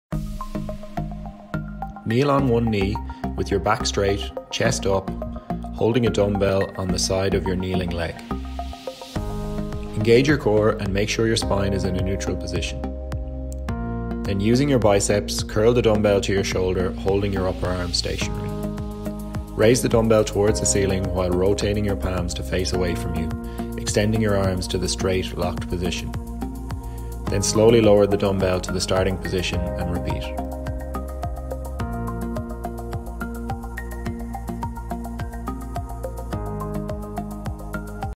Half Kneeling Dumbbell Curl and Press
1 / Kneel on one knee, with your back straight, chest up, holding a dumbbell on the side of your kneeling leg.
2 / Engage your core and make sure your spine is in a neutral position.
3 / Using your biceps, curl the dumbbell to your shoulder holding your upper arms stationary.
4 / Raise the dumbbell towards the ceiling while rotating your palms to face away from you, extending your arms to the straight, locked position.
5 / Slowly lower the dumbbell to the starting position and repeat.